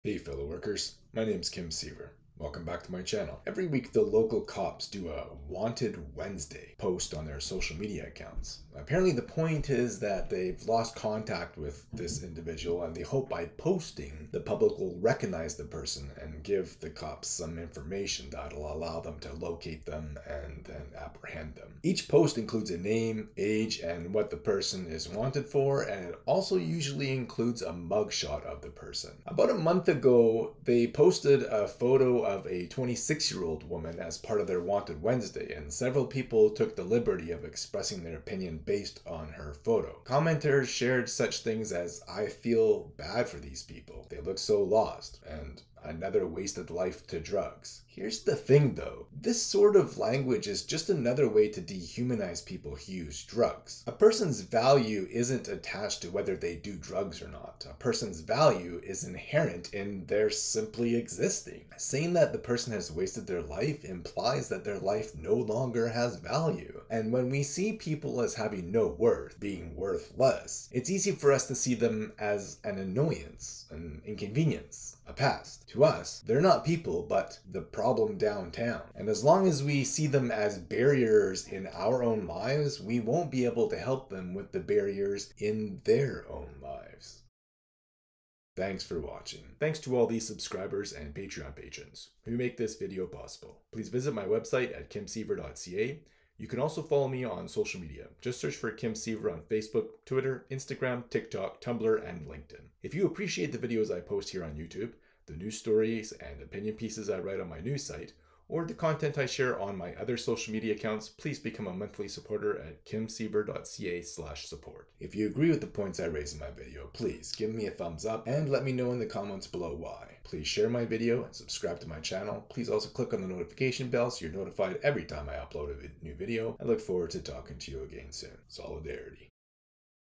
Why the Lethbridge Police Service’s “Wanted Wednesday” is messed up
Every week, the local cops do a “Wanted Wednesday” post on their social media accounts.